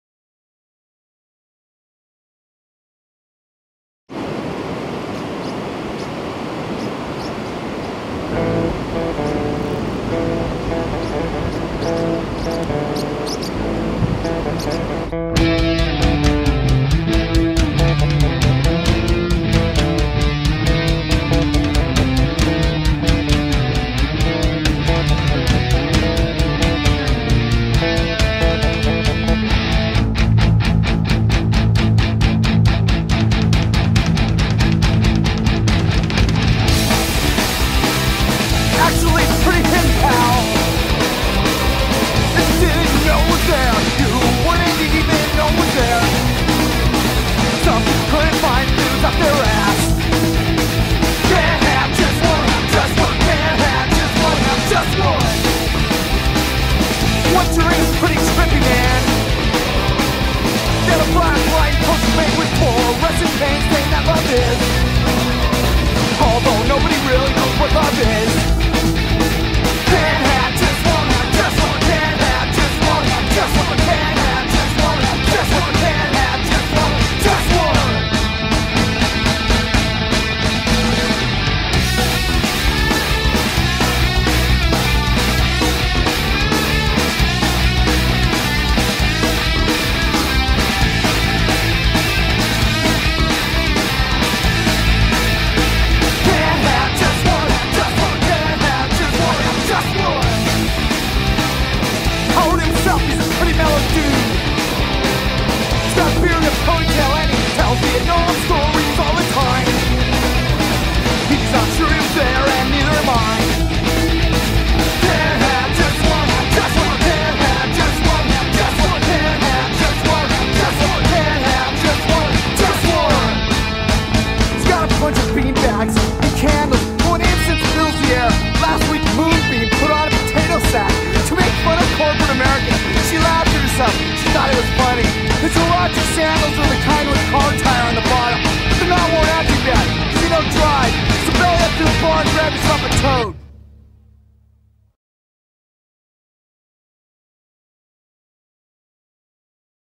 2-26-12_Ecuador.mov
First Edit of Ecuador video, Ecuador surf, music by White Kaps